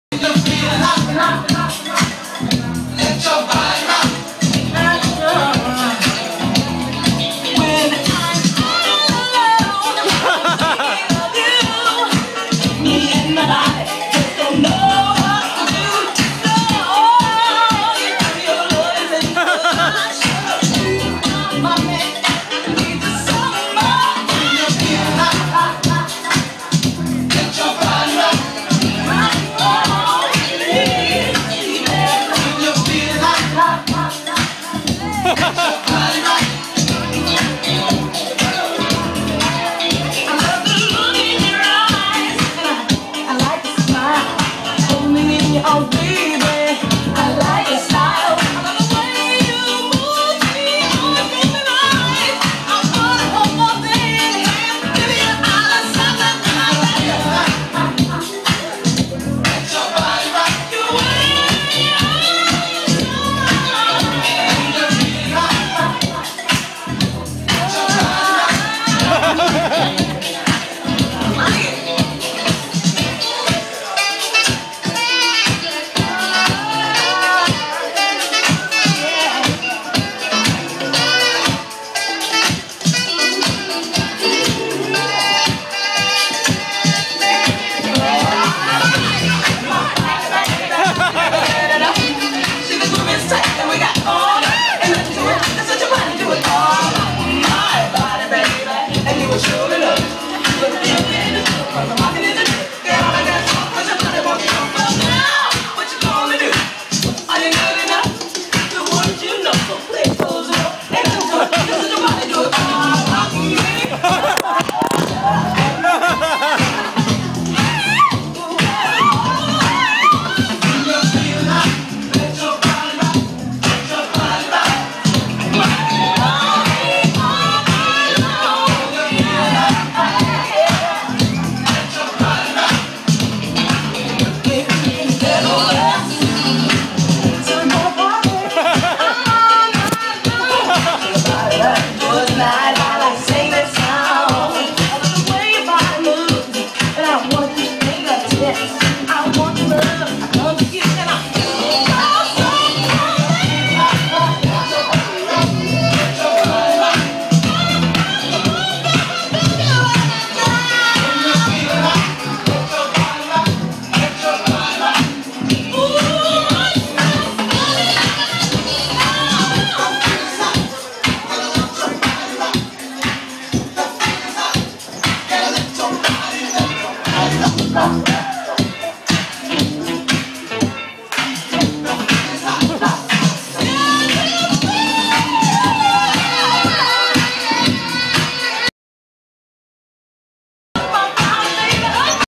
Miss Kristins Soul Train Birthday Party
All the beautiful people were there.
Mixx Bar. Kailua Kona, Hawaii. 5/21/11